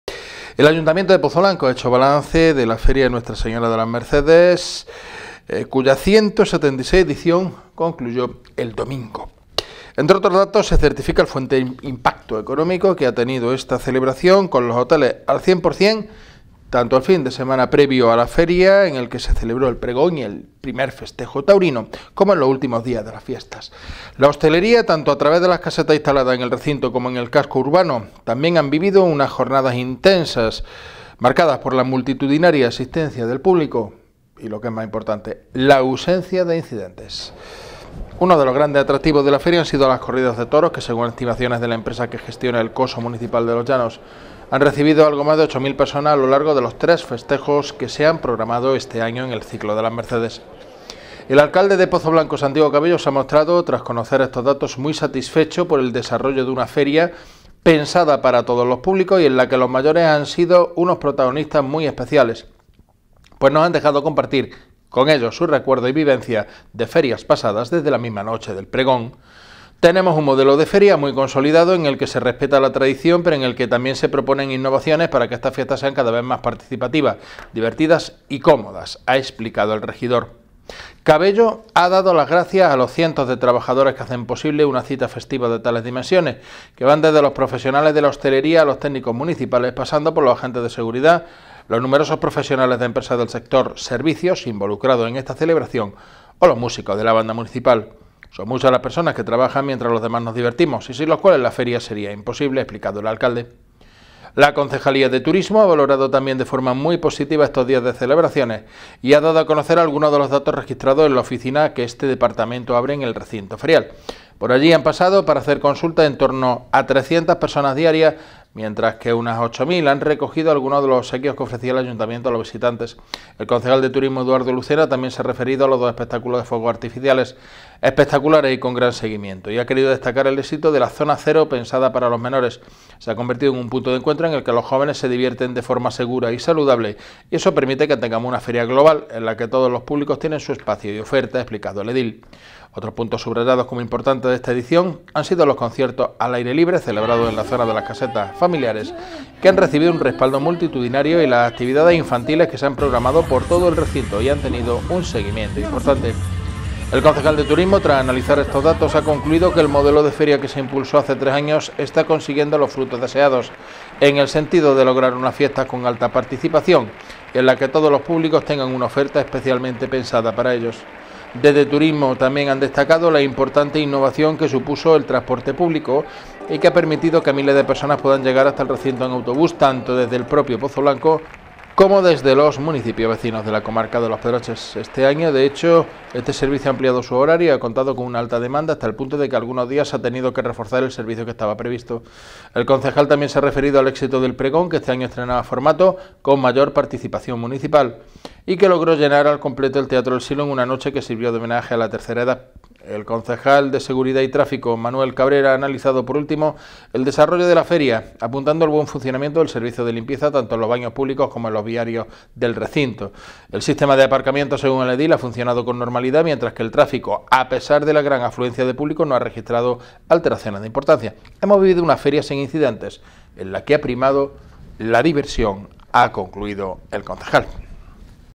La Feria deja un fuerte impacto económico en Pozoblanco
El Ayuntamiento de Pozoblanco ha hecho balance de la Feria de Nuestra Señora de las Mercedes, cuya 176 edición concluyó el domingo. Entre otros datos, se certifica el fuerte impacto económico que ha tenido esta celebración, con los hoteles al cien por cien tanto en el fin de semana previo a la feria, en el que se celebró el pregón y el primer festejo taurino, como en los últimos días de las fiestas. La hostelería, tanto a través de las casetas instaladas en el recinto como en el caso urbano, también ha vivido jornadas  intensas, marcadas por la multitudinaria asistencia de público y la ausencia de incidentes. Uno de los grandes atractivos de la Feria han sido las corridas de toros, que, según estimaciones de la empresa que gestiona el coso municipal de Los Llanos, ha recibido algo más de 8.000 personas a lo largo de los tres festejos que se han programado este año en el ciclo de Las Mercedes.